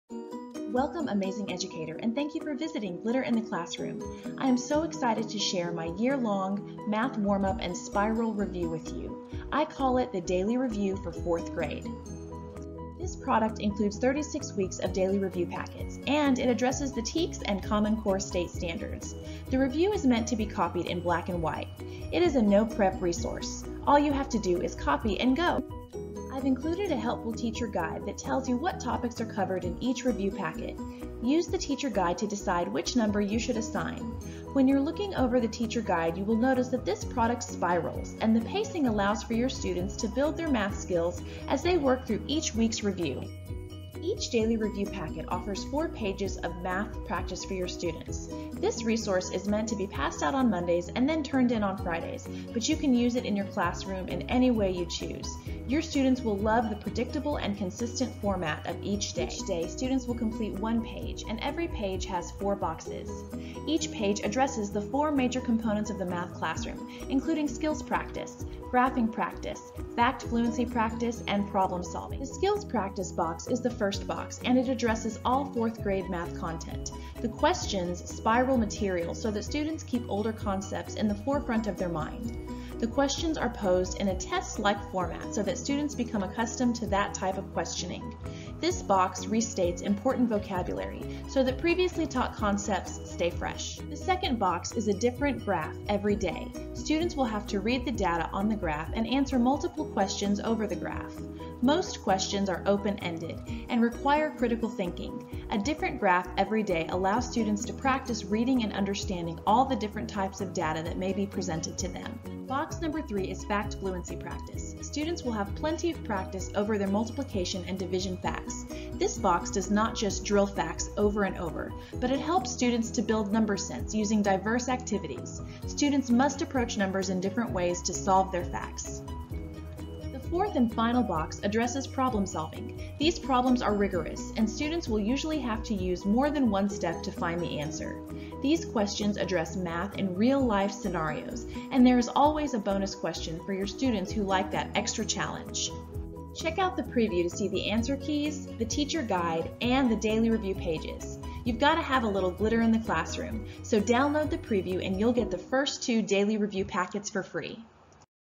4th Grade Daily Math Warm Up-ENTIRE YEAR-Daily Review-Math Spiral Review
This 4th grade daily math warm up and spiral review gives your students 4 boxes a day to complete which cover problem solving, graphing, fact fluency practice, and a different spiraled math skill. This product includes an ENTIRE YEAR (36 weeks) of reviews! JUST COPY AND GO!